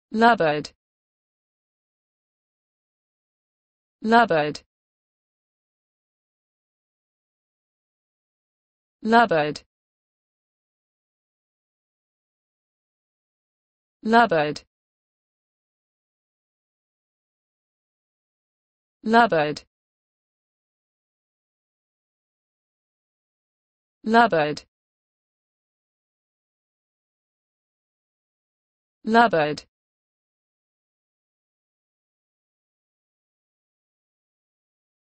the correct pronunciation of lucency in English.
This video shows you how to pronunciation lucency in English.